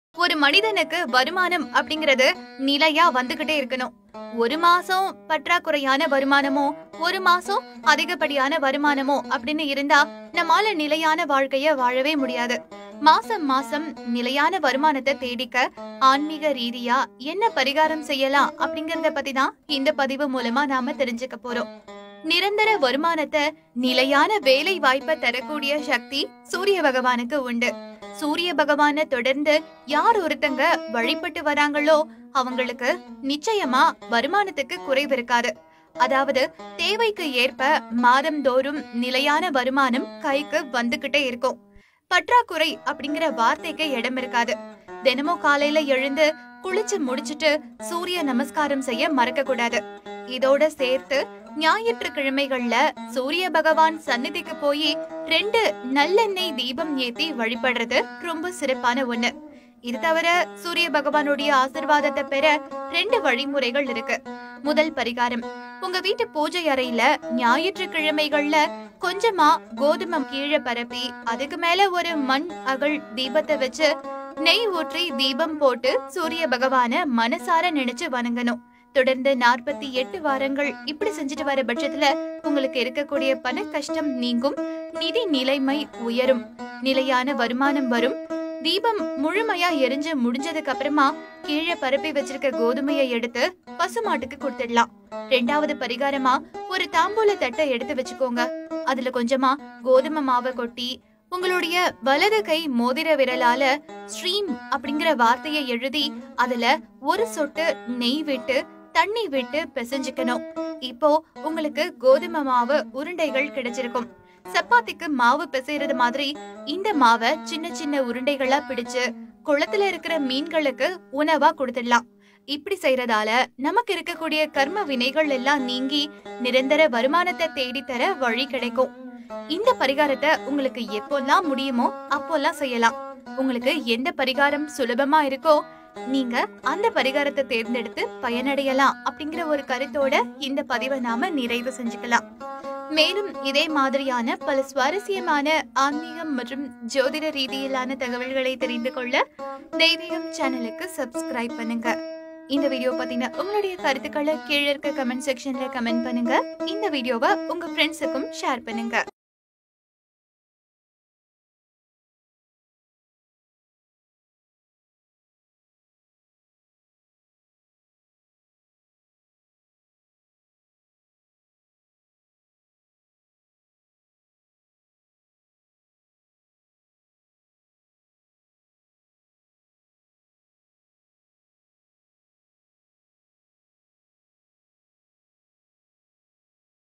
உங்களுக்கு நிலையான வருமானம் கிடைக்க இவரை இப்படி வழிபடுங்கள்.
மாதம்தோறும் நிலையான வருமானம், நிரந்தரமாக வந்துகொண்டே இருக்க இவரை வழிபட்டாலே போதும்.